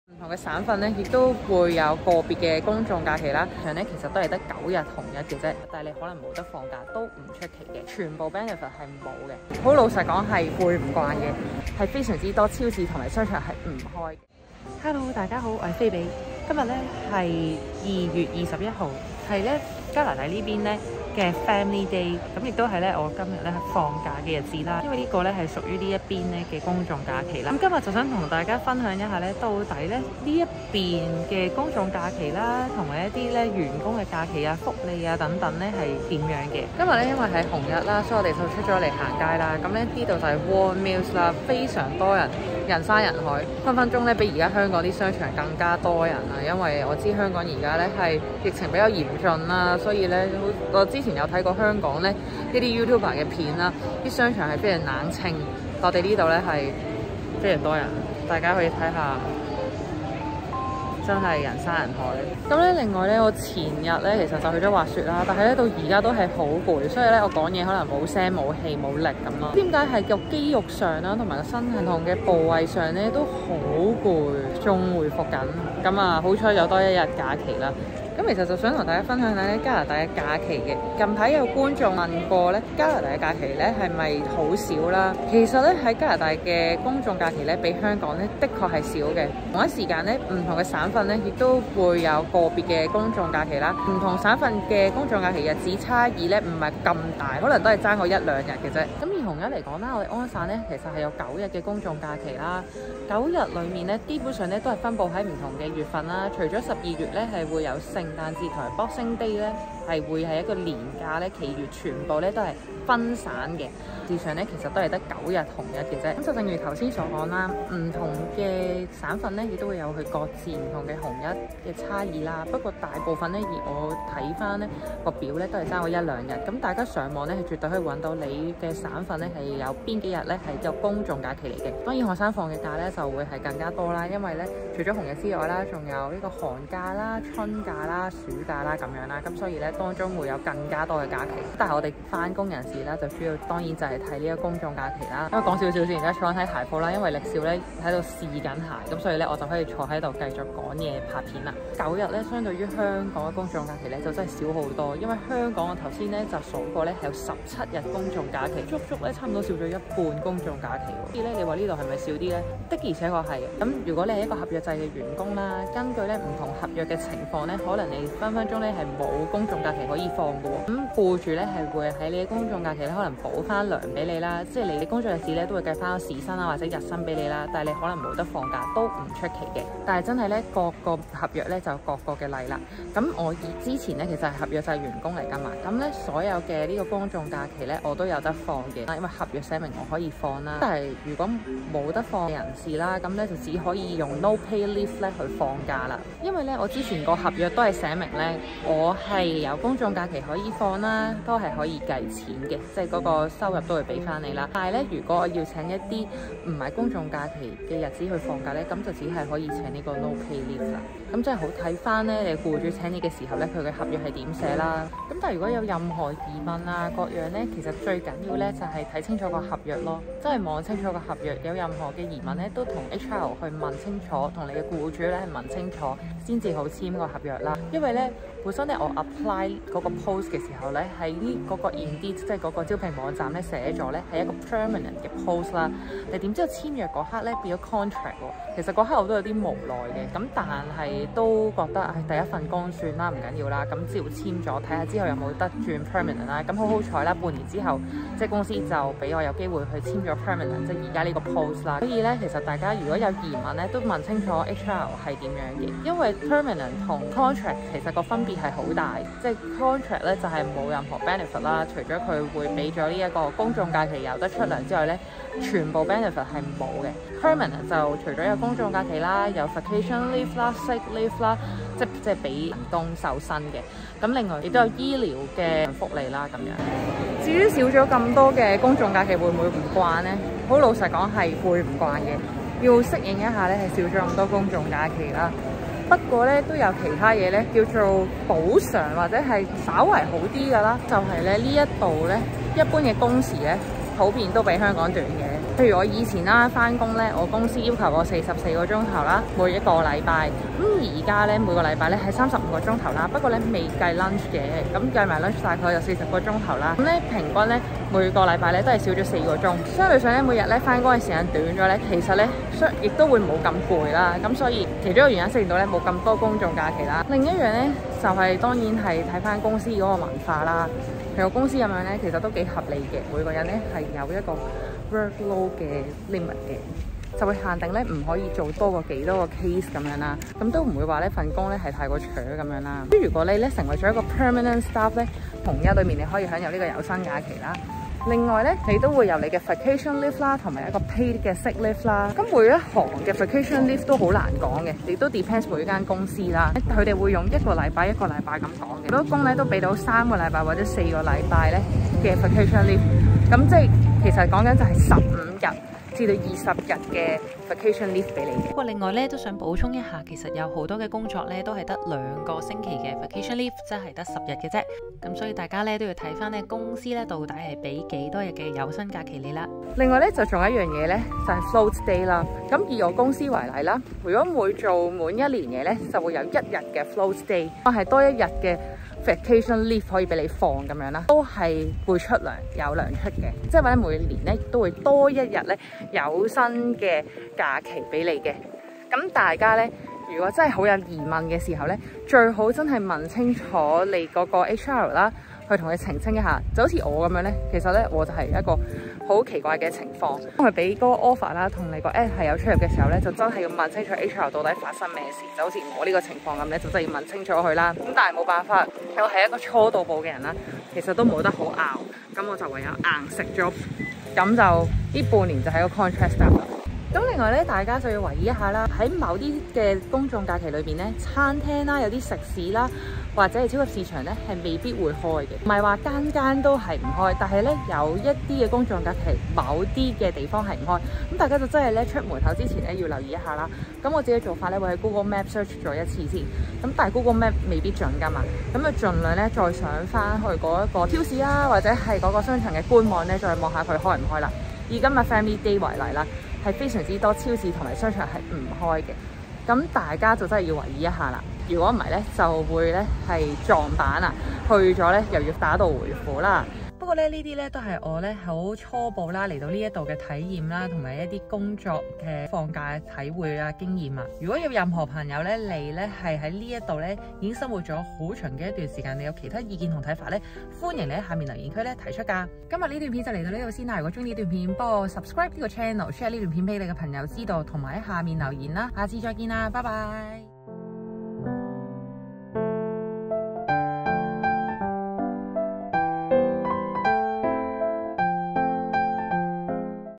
[移民加拿大] 🇨🇦 加拿大公眾假期比香港少一半!?｜恆常職位變合約制😠!?｜公眾假期很多商店不營業?｜多倫多生活｜加拿大移民｜菲比VLOG
今次分享一下加拿大公眾假期與香港的分別
以及加拿大員工可享有的假期福利